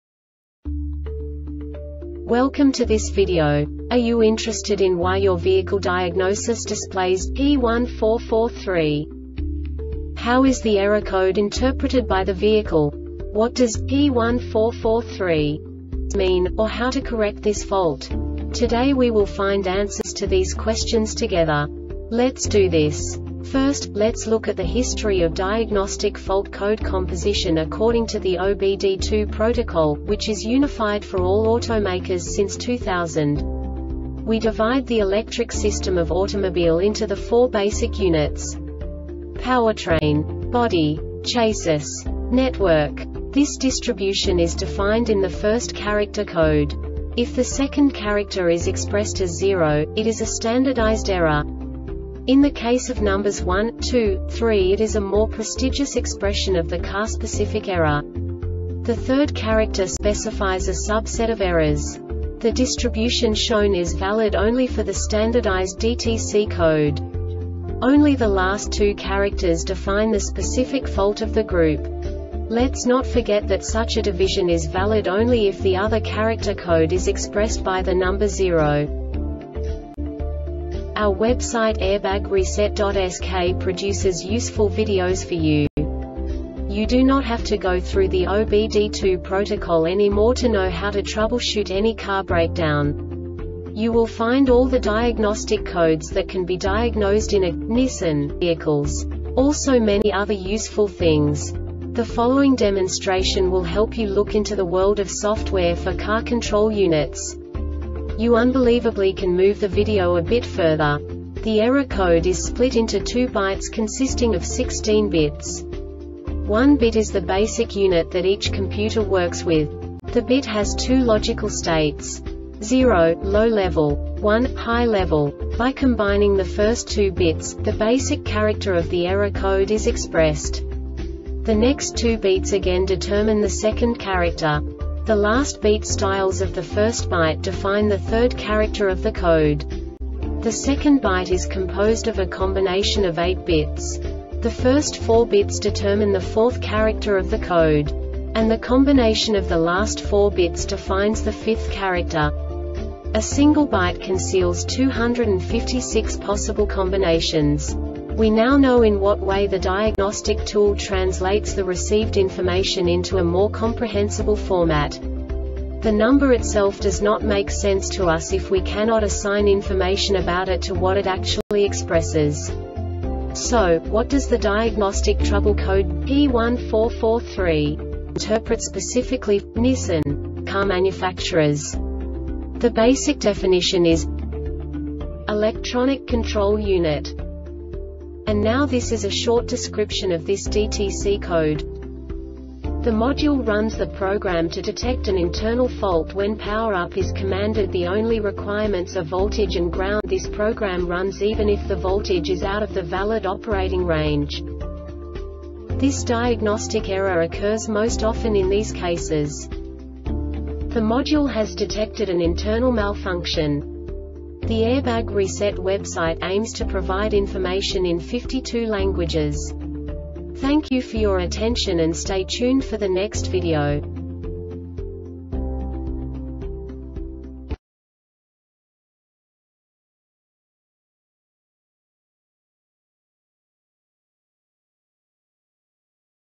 DTC Nissan P1443 Short Explanation
Contents:
0:21 Basic DTC analysis according to OBD2 protocol standard.
1:48 Insight into programming
2:58 A diagnostically understandable description of the Diagnostic Trouble Code
3:05 Basic error definition

Make:
Nissan
Code:
P1443
Definition:
Electronic Control Unit
Description: